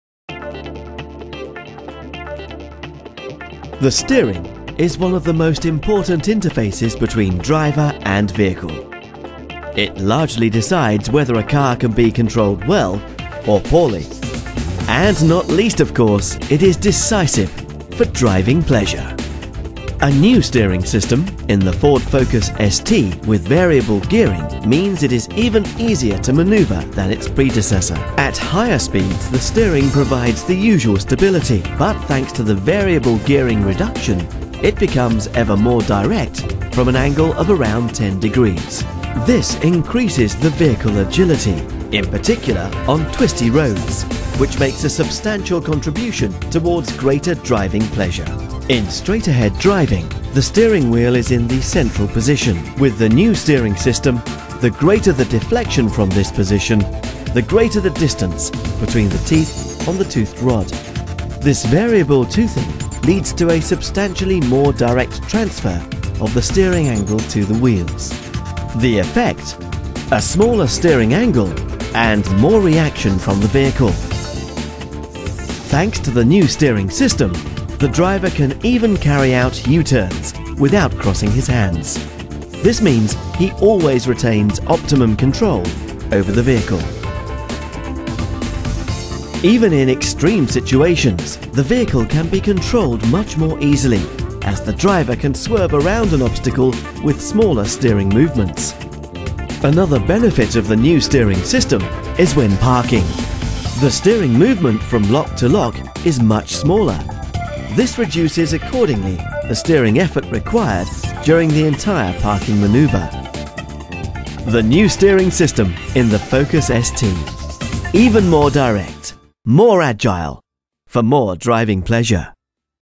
Ford Focus ST 2012: come funziona lo sterzo della nuova Focus
Il sistema di sterzo è del tipo adattivo, con una sensibilità minore in rettilineo, a tutto vantaggio della stabilità di marcia, mentre all'aumentare dell'angolo volante corrisponde una risposta più fedele e precisa.